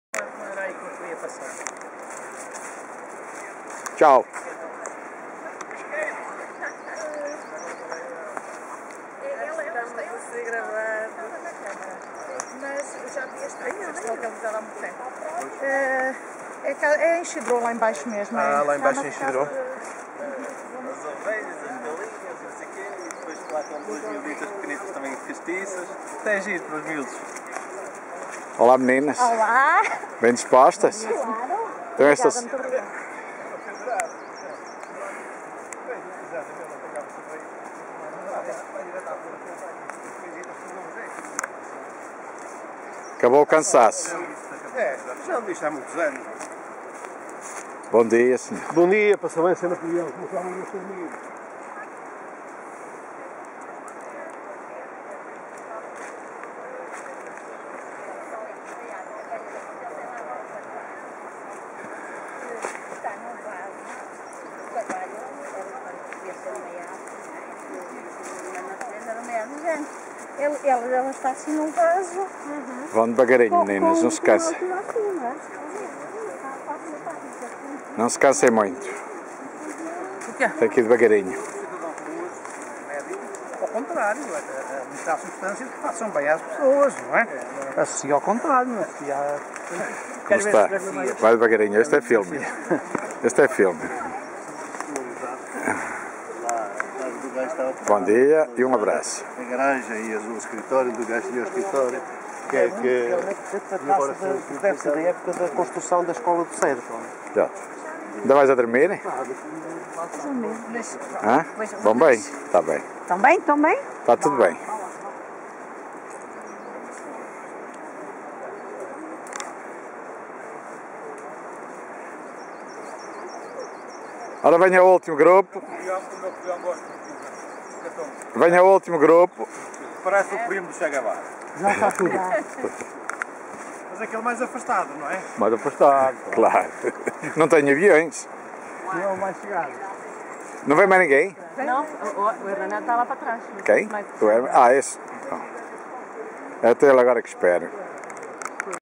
Os caminheiros na natureza Vale do Bestança
Cinfães Bestança 2013 Caminhada da Primavera Video de Napoleão Monteiro